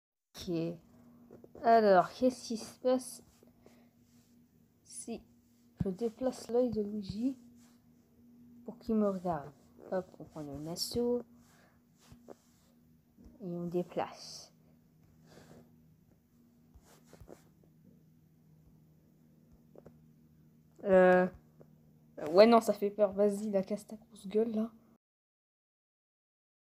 PIRE IDÉE EVER😭 ( j'ai essayer de faire que luigi me regarde et.... )
en vrai, il est cute avec ses lunettes

tu me dira, lisa craque PTDR